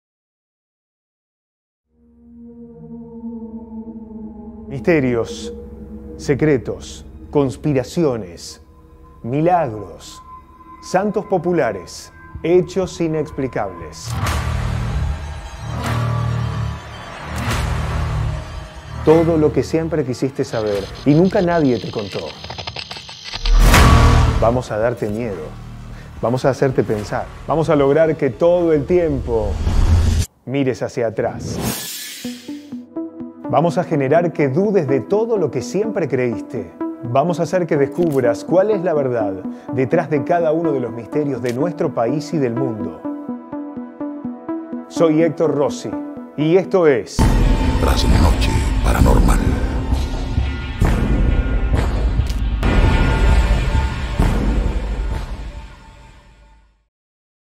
¡Se viene Trasnoche Paranormal en Tv!
En 2020, vas a poder vernos. Dejá tu comentario acá abajo y compartí este video.